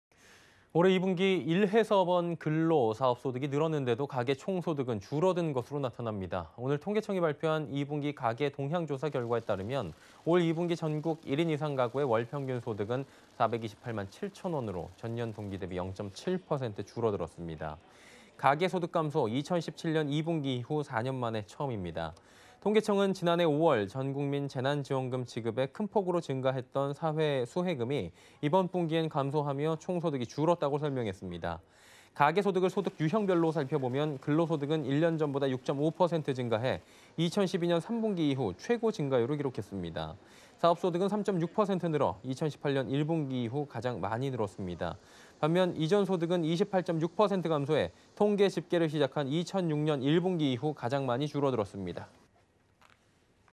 근로·사업소득 늘었는데…가계 총소득 감소
올해 2분기 ‘일해서 번’ 근로·사업소득이 늘었는데도 가계 총소득은 줄어든 것으로 나타났습니다.

오늘 (19일) 통계청이 발표한 ‘2분기 가계동향조사 결과’에 따르면 올해 2분기 전국 1인 이상 가구 월평균 소득은 428만7,000원으로 전년동기대비 0.7% 감소했습니다. 가계 소득 감소는 2017년 2분기(-0.5%) 이후 4년만에 처음입니다.

통계청은 “지난해 5월 전국민 재난지원금 지급에 큰 폭으로 증가했던 사회수혜금이 이번 분기에는 감소하면서 총소득이 줄었다”고 설명했습니다.

가계소득을 소득 유형별로 살펴보면, 근로소득(274만3,000원)은 1년 전보다 6.5% 증가해 2012년 3분기(6.9%) 이후 최고 증가율을 기록했습니다. 사업소득(80만6,000원)은 3.6% 늘어 2018년 1분기(3.7%) 이후 가장 많이 늘었습니다.

반면 이전소득(61만7,000원)은 28.6% 감소해 통계 집계를 시작한 2006년 1분기 이후 가장 많이 줄었습니다./